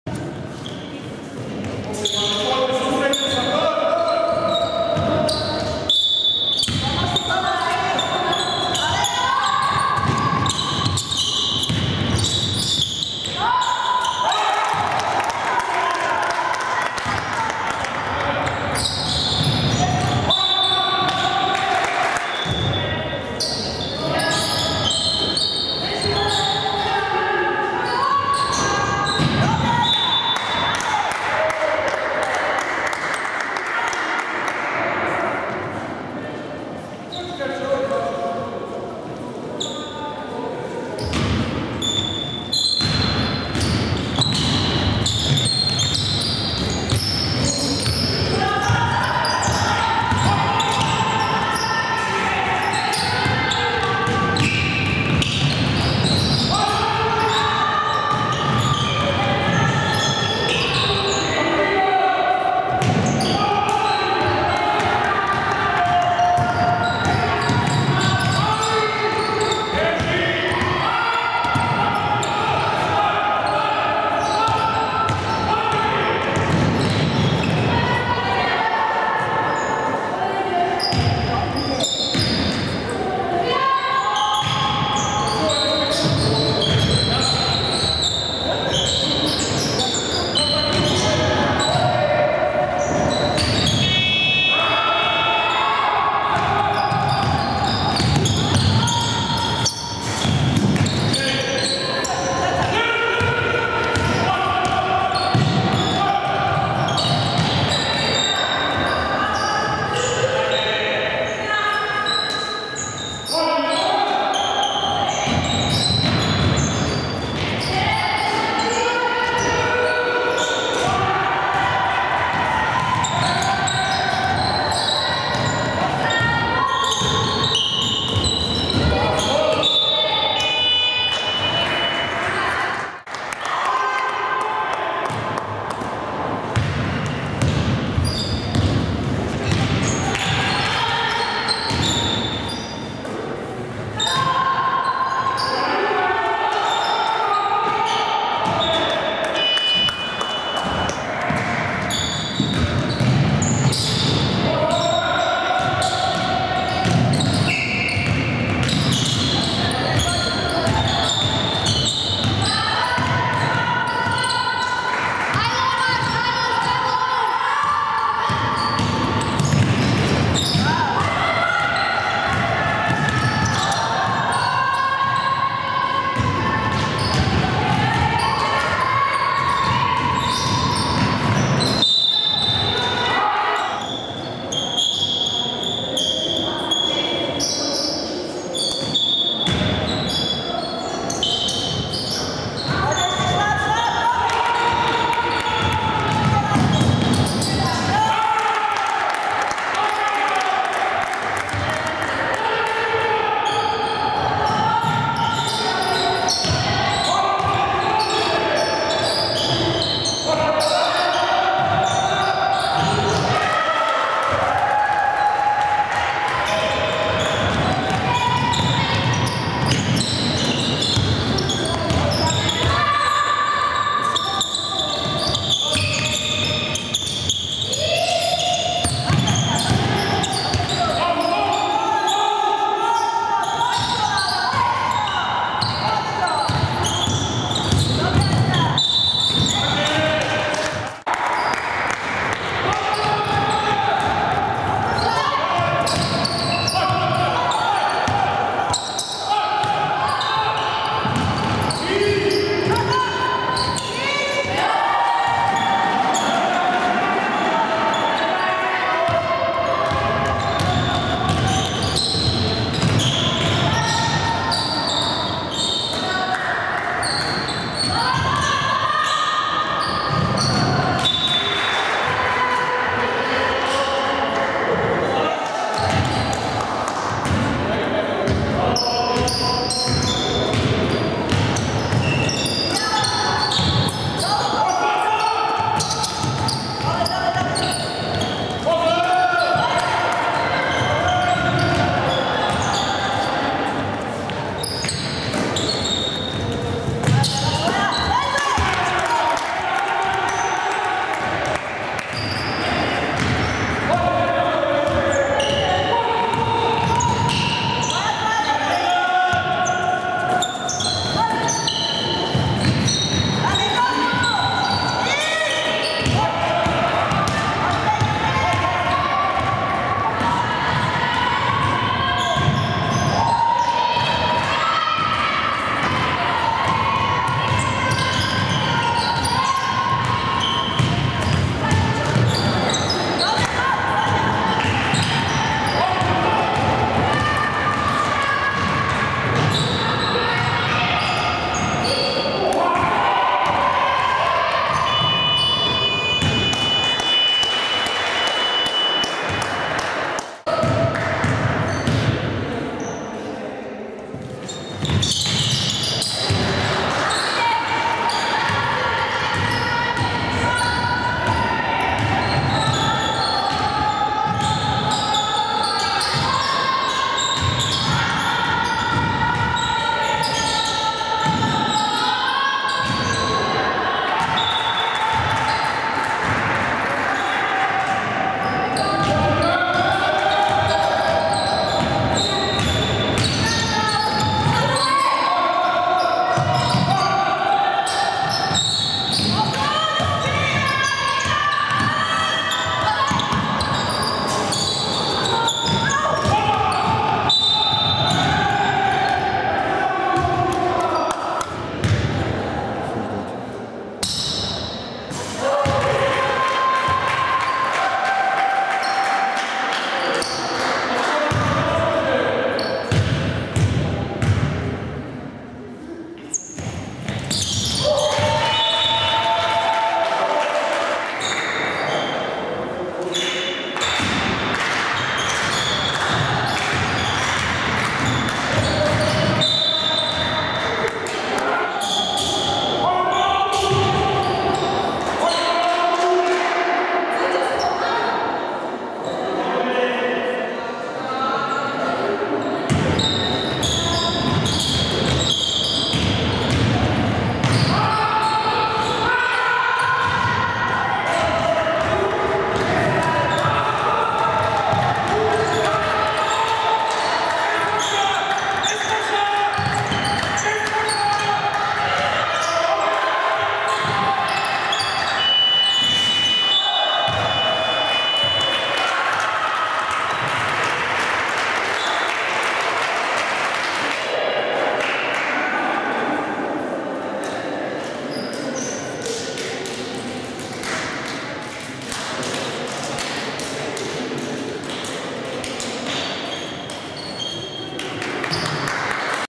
BK ZŠ ŠKP Zvolen - BK EILAT PU Prešov 54:49
V poslednú februárovú nedeľu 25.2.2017 sa v športovej hale na zvolenskom sídlisku Západ hral basketbalový zápas už 6. kola 1. ligy žien.